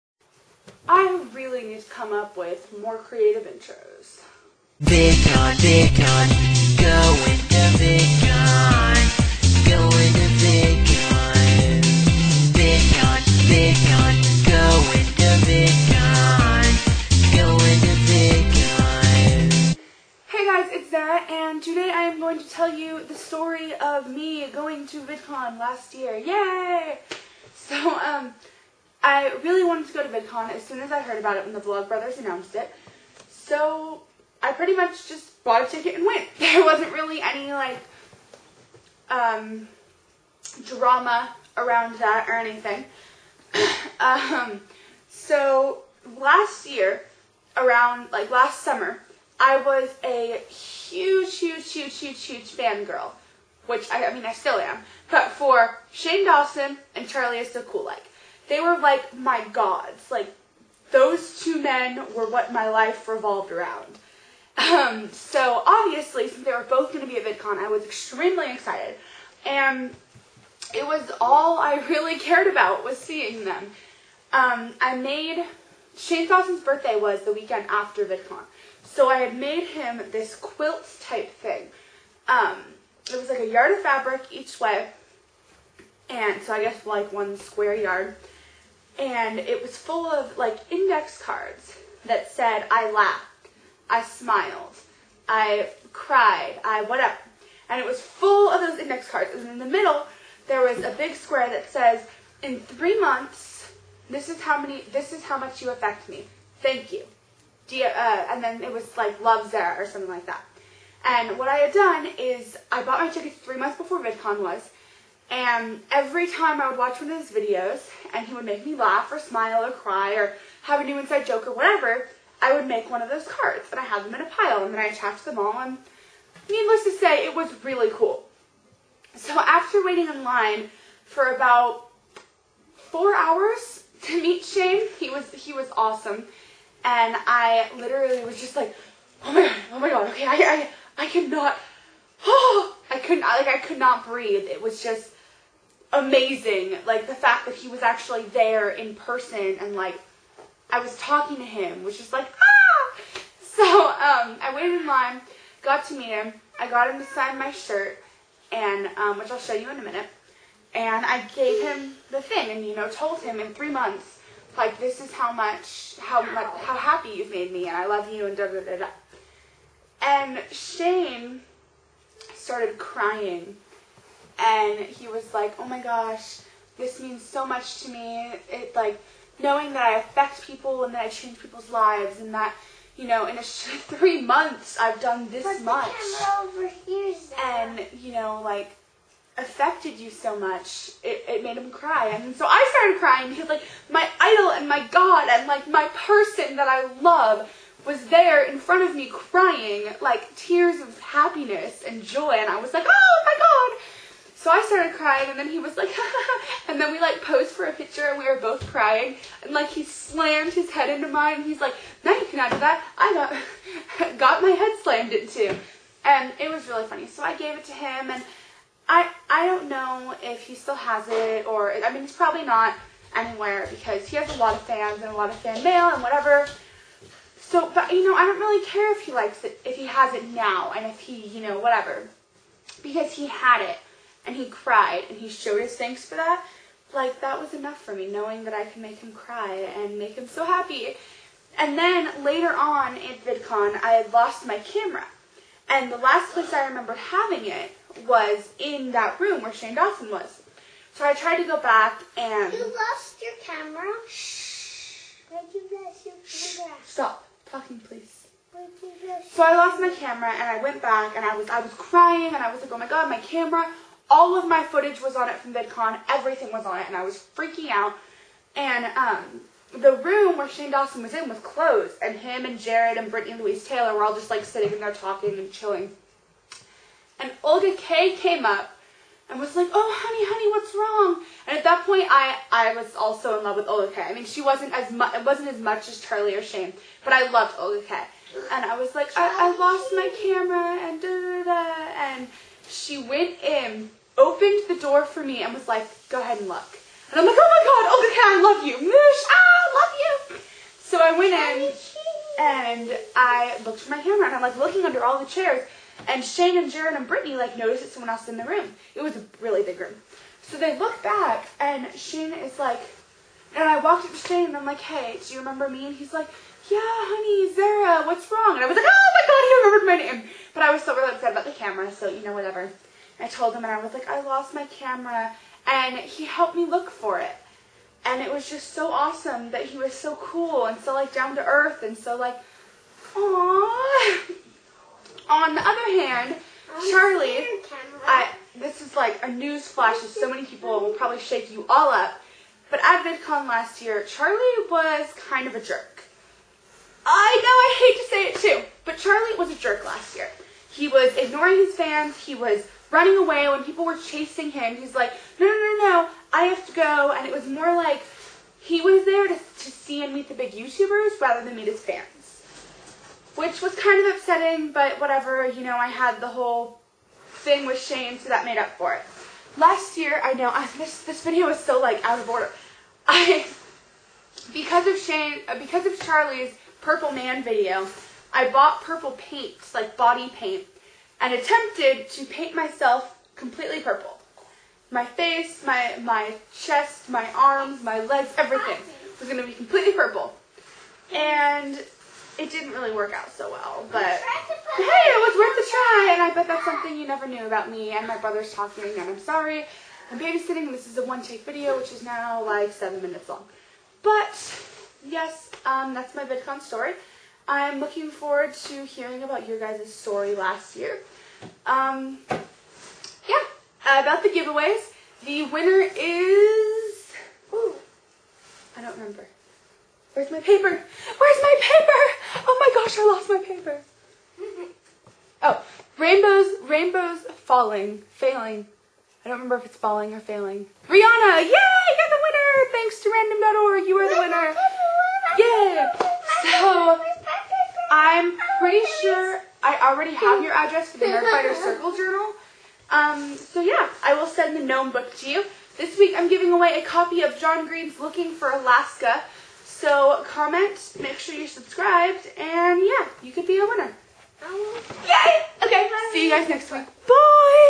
Zaira was a FAN GIRL at VidCon 2010 and GIVEAWAY!
Sorry the video is so long...
LINKS:
My channel:

Our vYou! (if you want a specific one of us to answer a question, please direct it to that person)

Our Dailybooth!